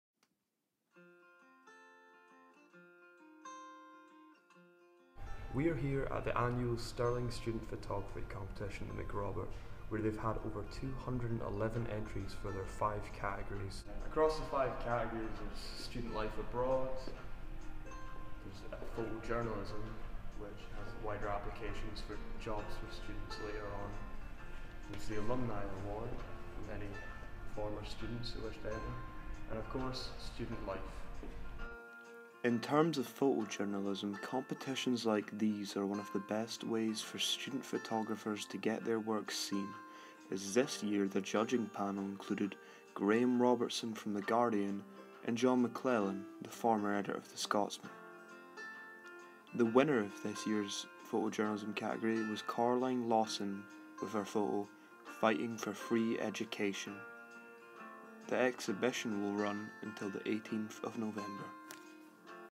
Macrobert showcases winners of Stiling University photo competition
The winning entries of Stirling University's photography competition are now on display in Macrobert arts centre